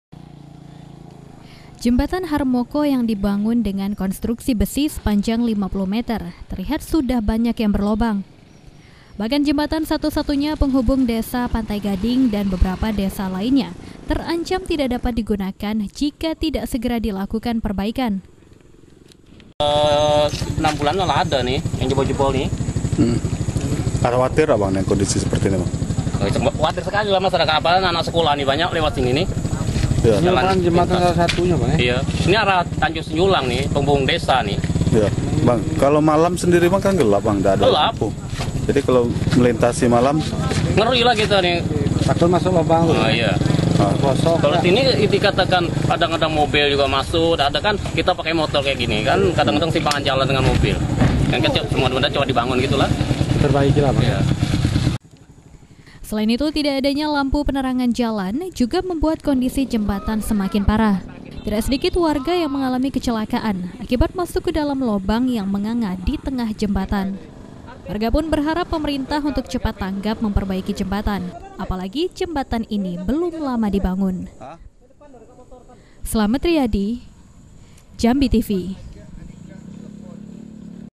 Jembatan Harmoko Di Tanjabbar Rawan Kecelakaan
Jembatan Harmoko yang dibangun belum sampai lima tahun terlihat sudah mulai rusak. Bahkan lantai jembatan sudah banyak yang bolong dan membahayakan pengguna jalan.

Facebook.com: Jambi-TV
Youtube.com: JambiTV Kebangaan Kito